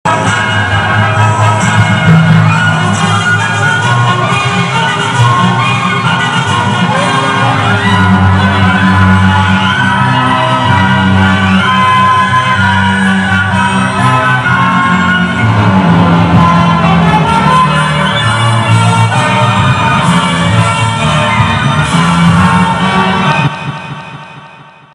pawtucket Rhode Island music RI orchestra 2008 september
this was a free outdoor concert with a archestra music in pawtucket ri.
it was excellent, enjoy the music .